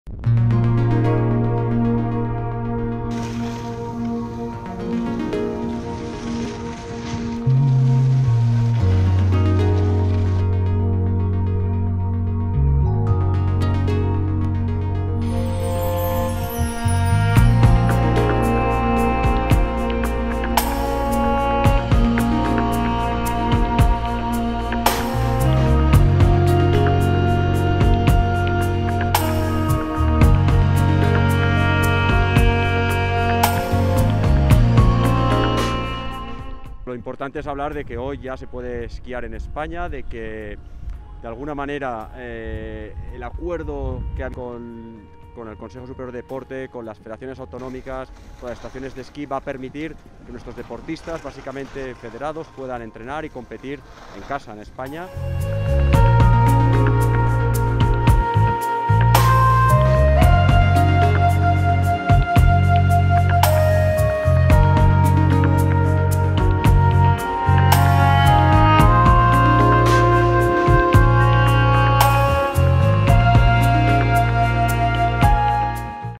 Skiing restarts in Catalonia, with Europe split on reopening slopes
🏂 While Europe remains divided over skiing restrictions, Catalonia has just reopened its slopes to winter sports fans.

🇪🇸 Spain now joins Austria and Switzerland as countries who are welcoming skiers back onto the slopes.